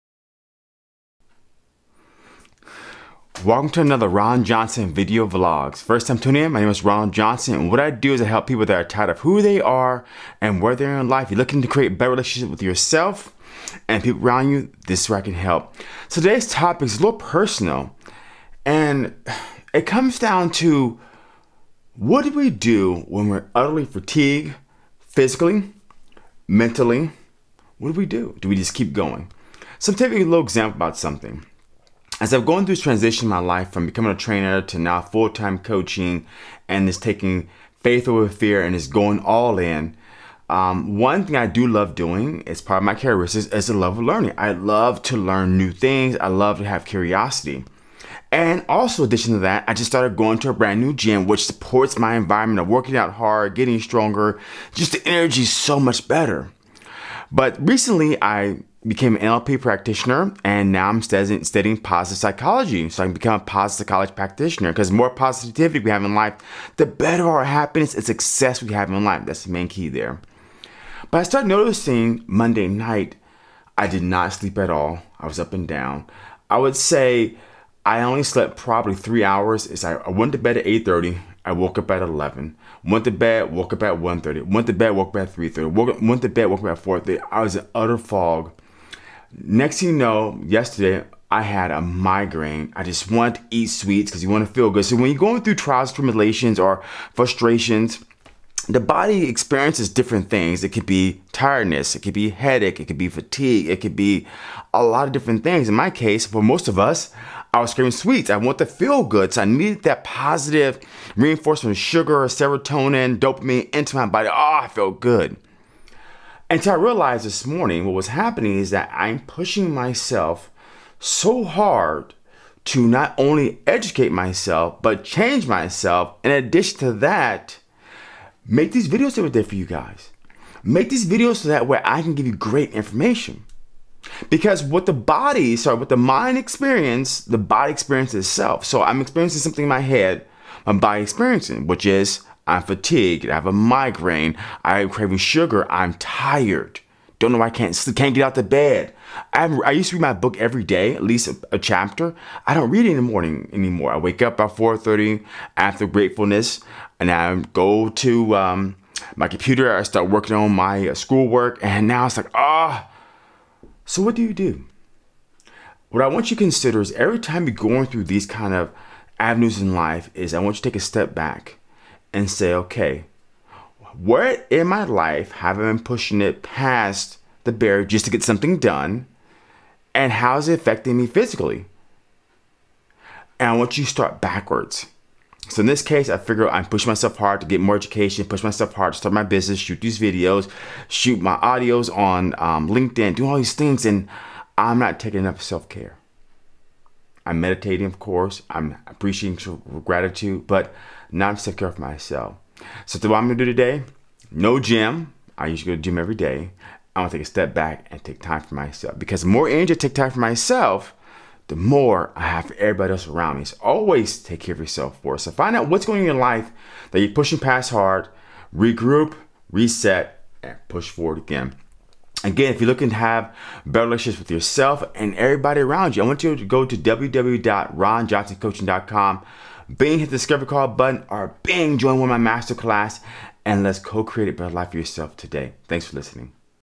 Mental Fatigue
How do you know when you're facing mental fatigue on the verge of burning out?  Most of the time we never recognize these signs until it's too late.  But, you do not have to wait until it's too late.
Listen to my video on what to do when are you facing mental fatigue. One last thing to consider is that the body experiences what's held in mind.